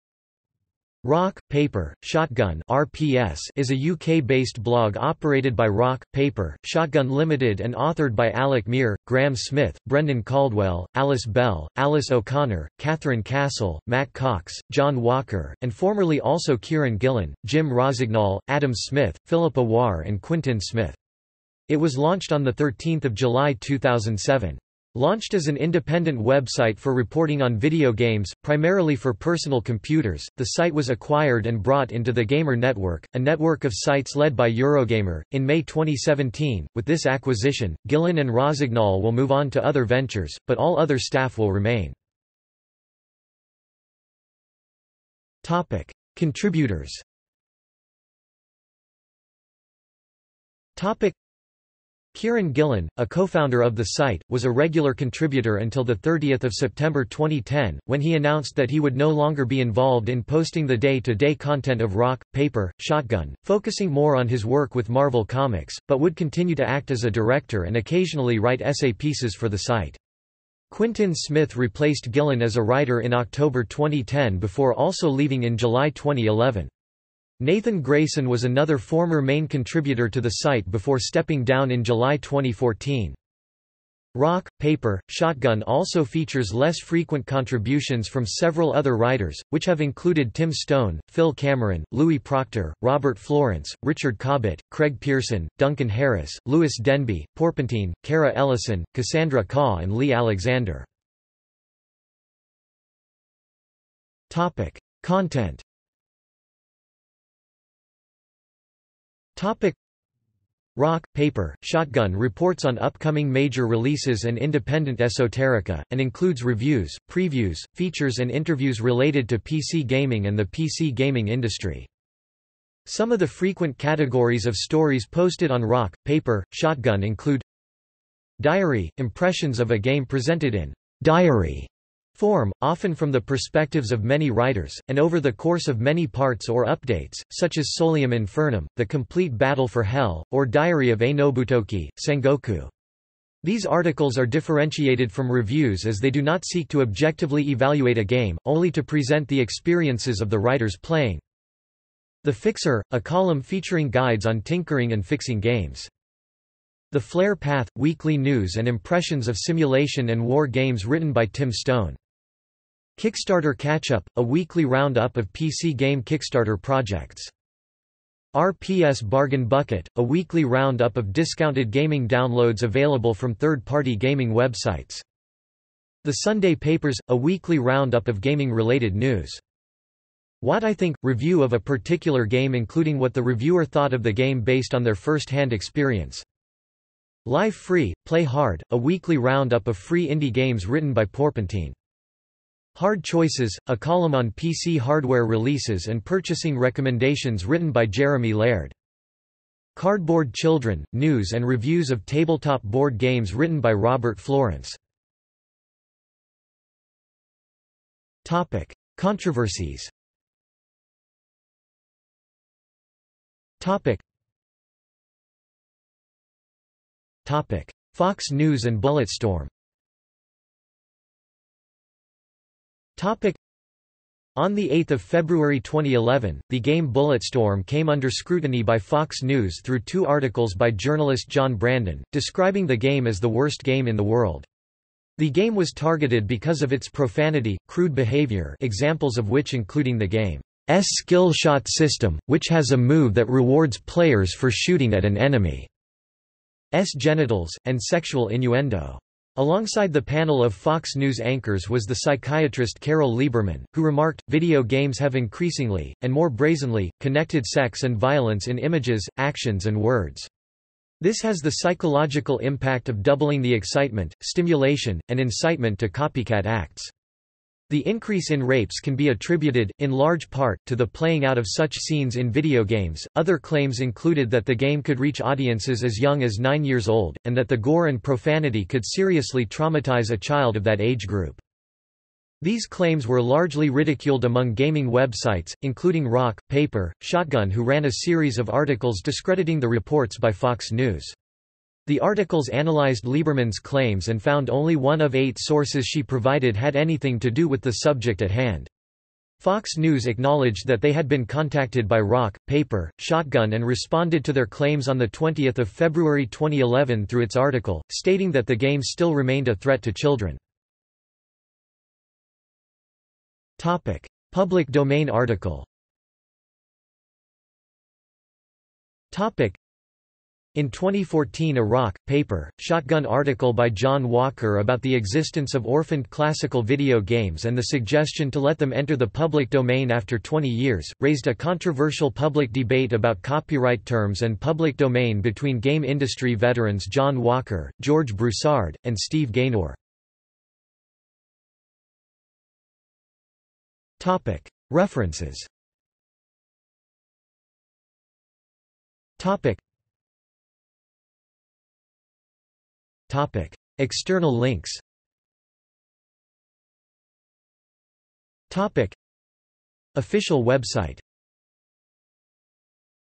Rock, Paper, Shotgun | Wikipedia audio article
This is an audio version of the Wikipedia Article:
Rock, Paper, Shotgun

SUMMARY
Rock, Paper, Shotgun (RPS) is a UK-based blog operated by Rock, Paper, Shotgun Ltd and authored by Alec Meer, Graham Smith, Brendan Caldwell, Alice Bell, Alice O'Connor, Katherine Castle, Matt Cox, John Walker, and formerly also Kieron Gillen, Jim Rossignol, Adam Smith, Philippa Warr and Quintin Smith. It was launched on 13 July 2007. Launched as an independent website for reporting on video games, primarily for personal computers, the site was acquired and brought into the Gamer Network, a network of sites led by Eurogamer, in May 2017; with this acquisition, Gillen and Rossignol will move on to other ventures, but all other staff will remain.